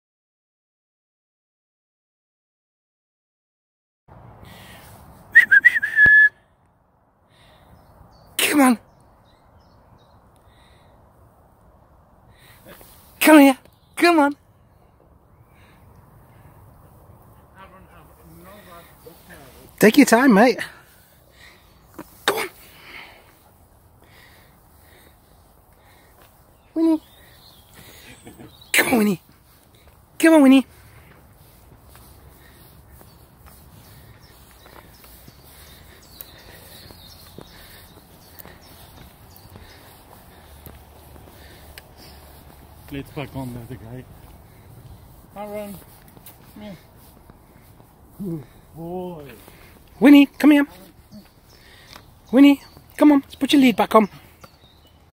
Craigslife - Winnie & Aaron in: Off the leash
Winnie and Aaron in lockdown walk off the leash. We don't normally let them walk off the leash as they can get too 'enthusiastic' when they see other dogs.
Aaron was knackered!